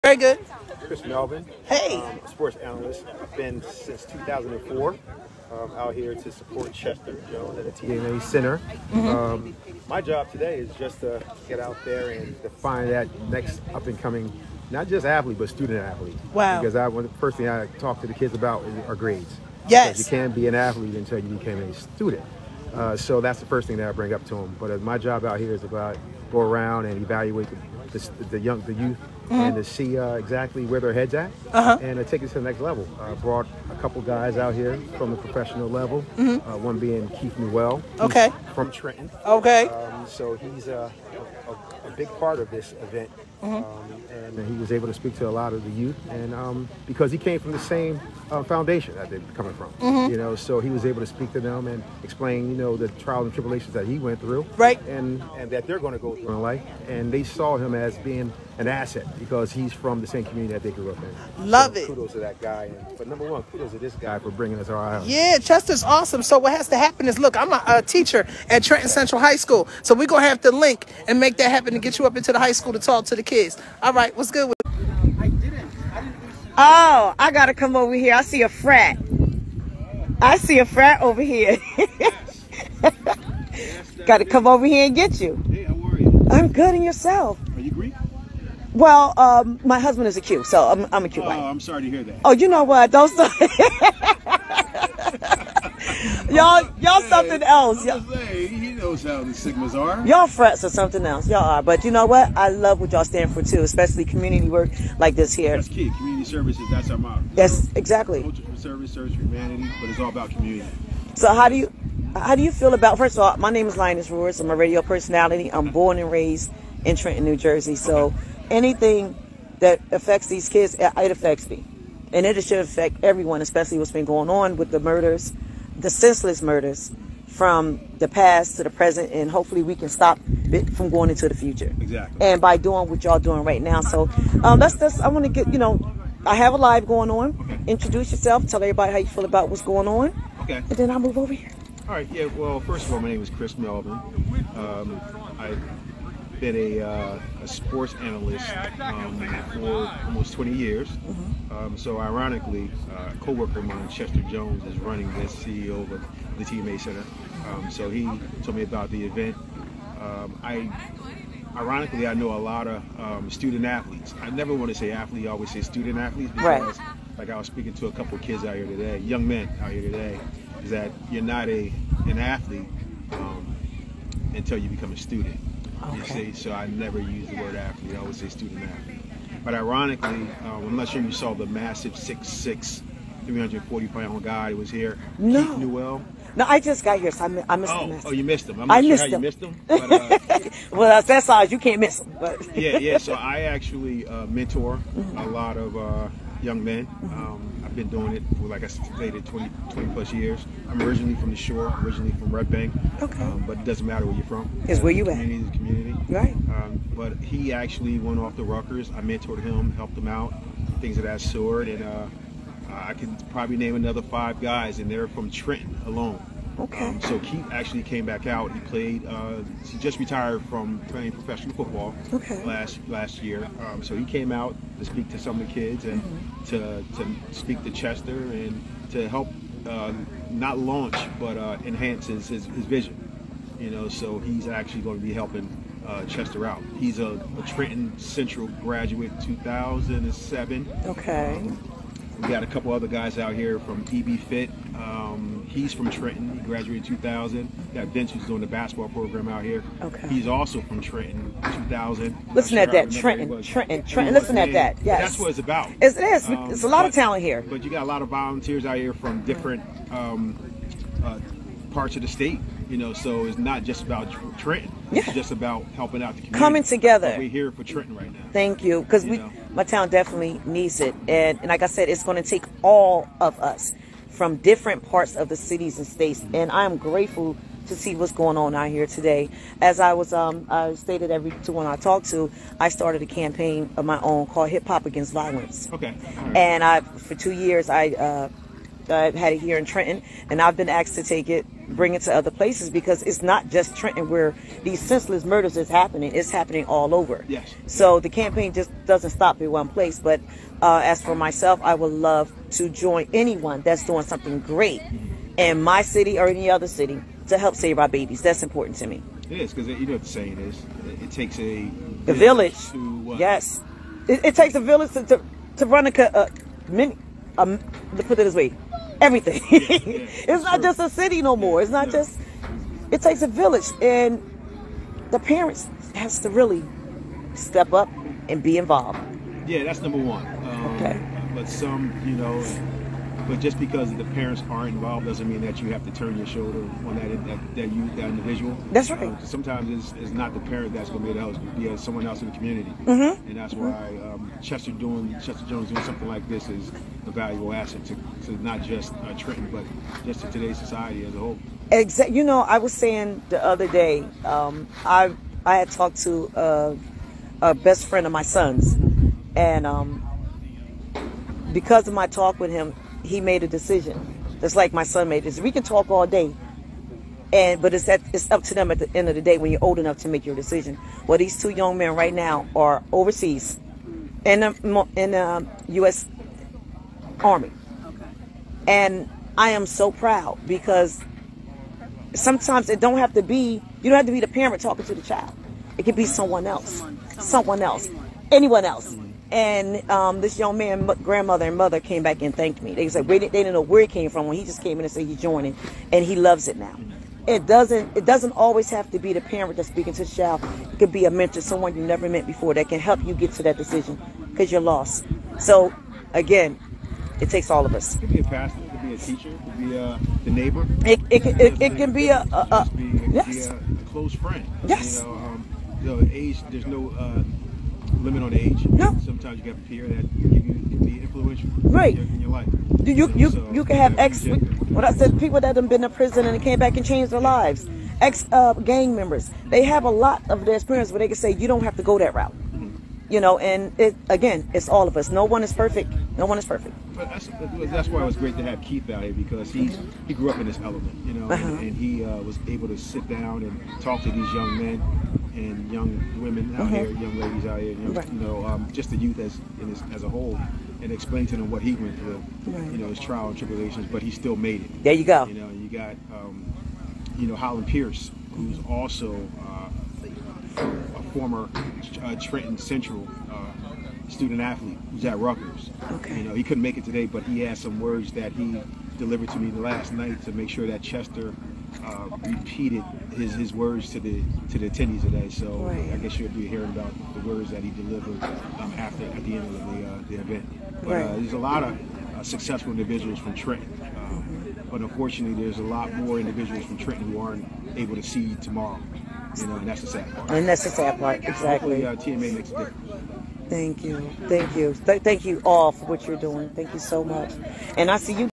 TMA Center of Trenton Training Day 2022 ... Chris Melvin Interview(s) 4/2/2022
The TMA Center of Trenton, NJ had its inaugural Training Day for the Community of Trenton.